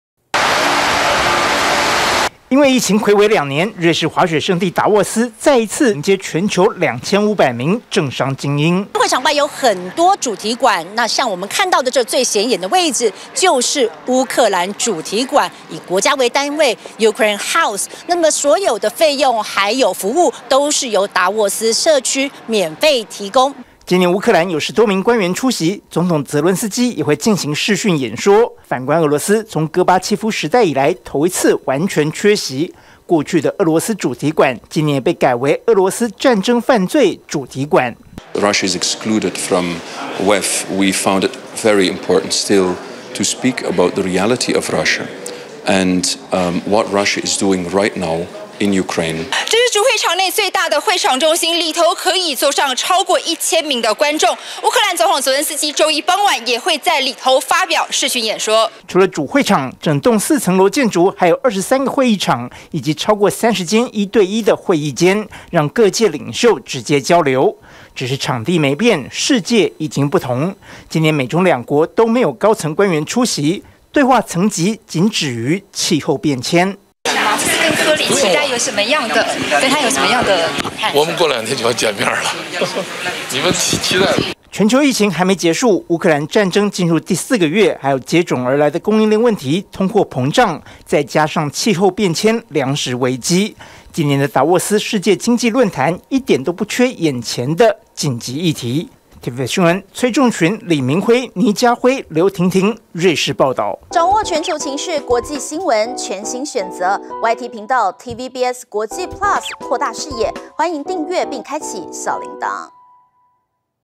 世界經濟論壇 設主題館揭俄戰爭罪行｜TVBS新聞
因為全球新冠疫情睽違兩年之後，達沃斯世界經濟論壇今年恢復實體舉辦，
結果又遇上Omicron疫情以及俄羅斯入侵烏克蘭，使得全球議題更加複雜，
TVBS採訪團隊，來自現場的第一手觀察。